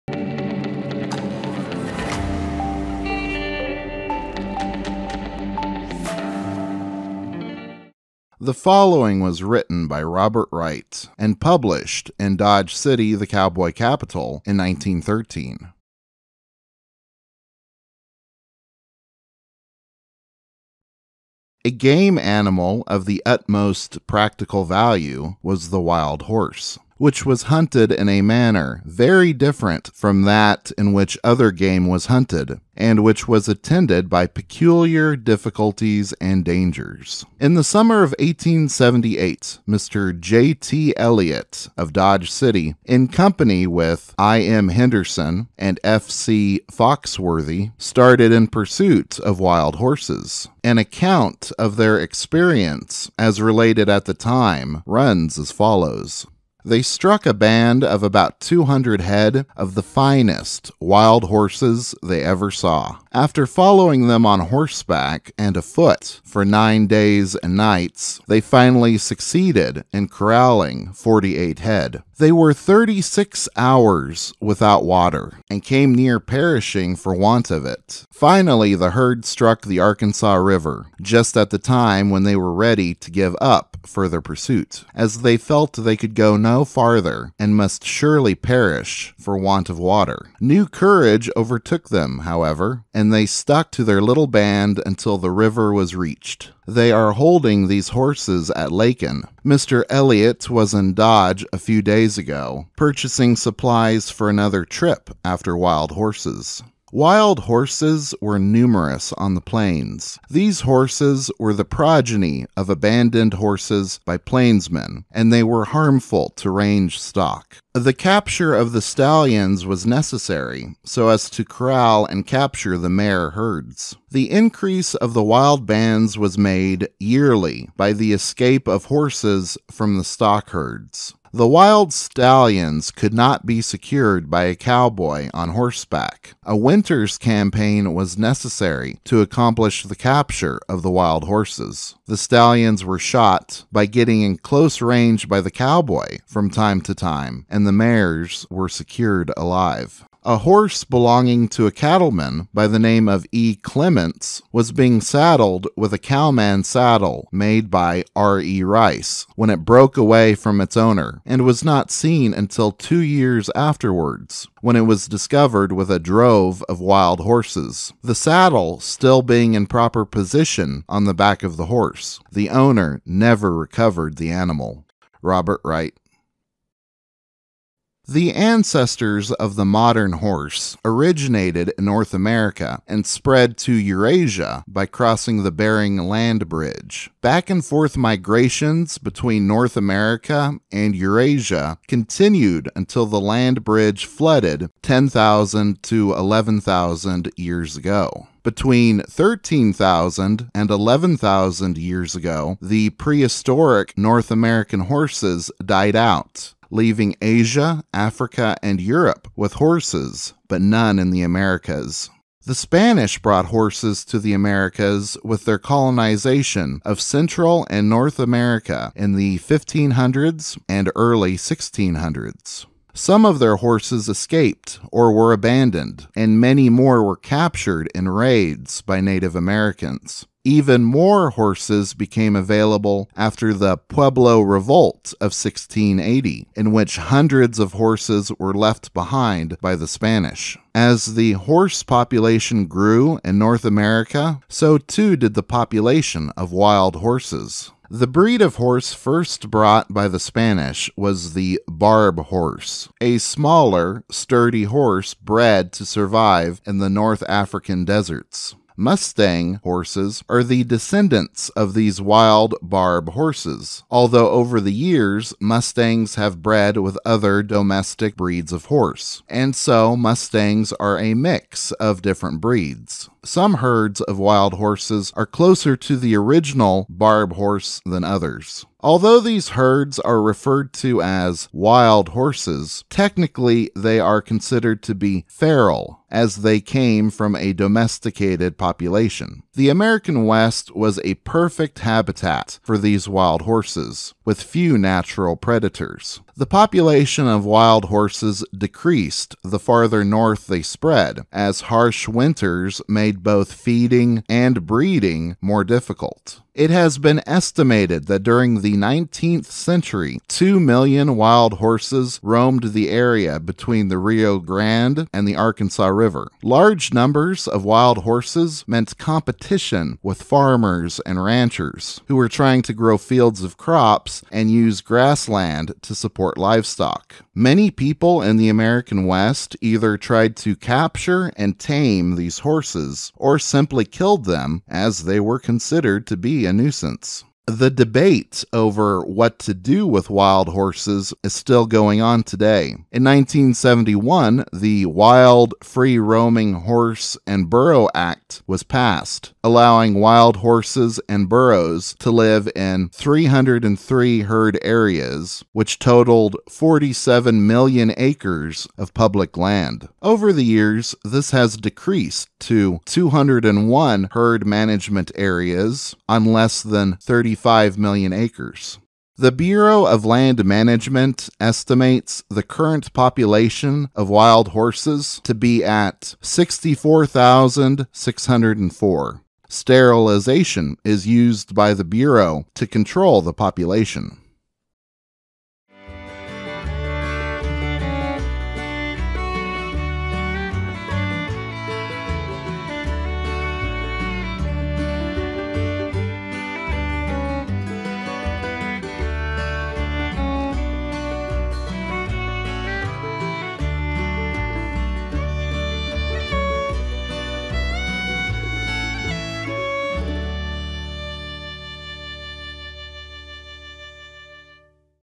Wild Horses in the Old West, Described by Robert Wright (Recollections)
In the wild west there were great herds of buffalo, herds of deer and herds of antelope, but what about the great herds of wild horses?  In this video learn from Robert Wright about people in the old west capturing wild horses.  Wild horses, often called "mustangs", are an important part of American frontier history.